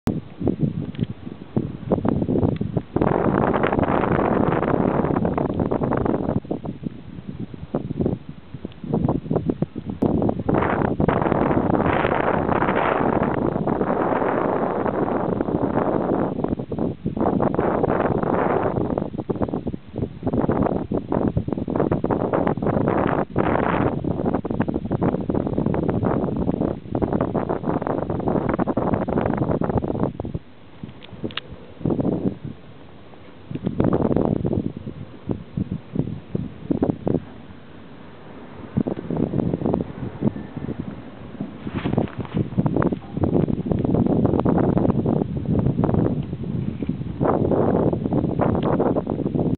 Snowing in Delfshaven, Rotterdam, 3 Feb 2012
First proper snow this winter!